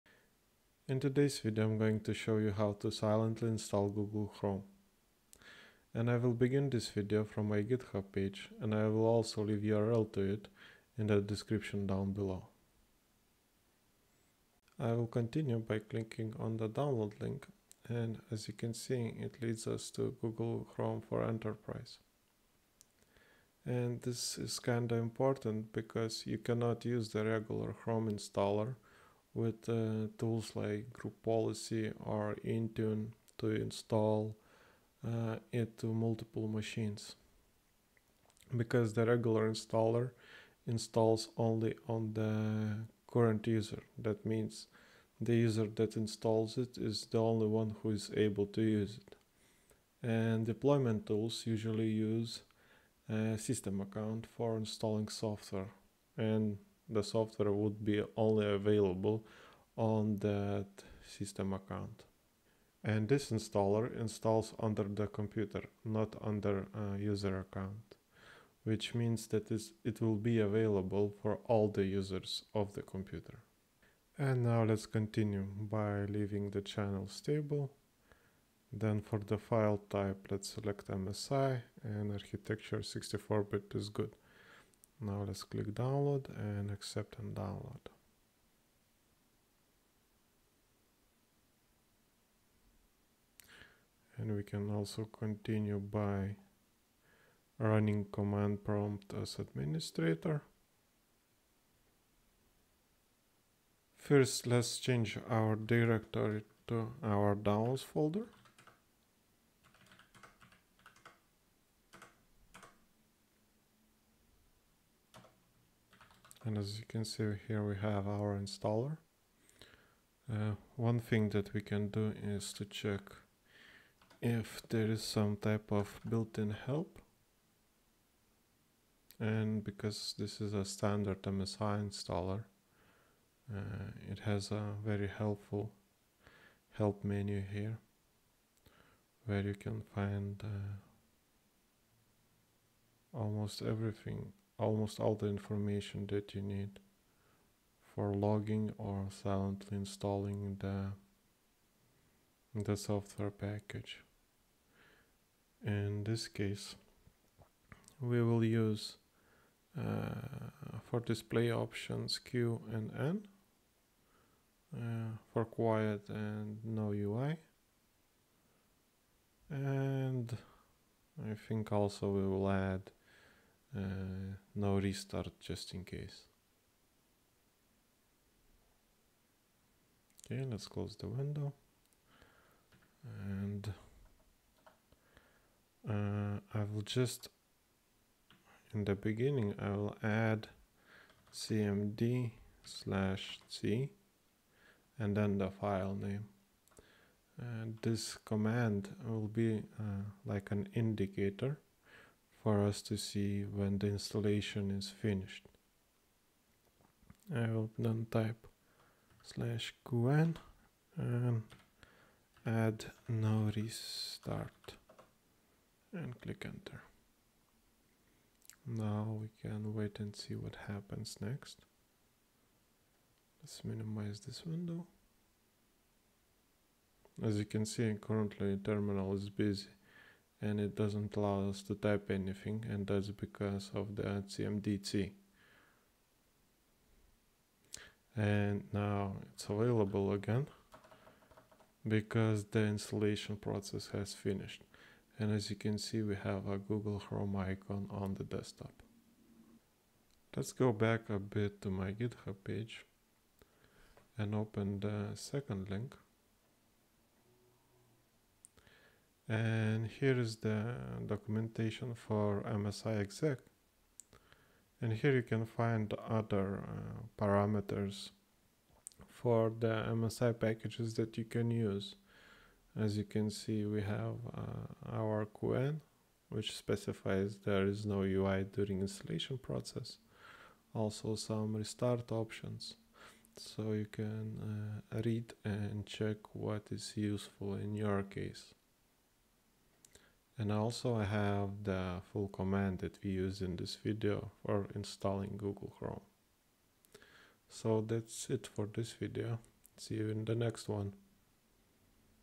Silent software installation: Google chrome
In this video I will show you how to silently install Google Chrome in Windows.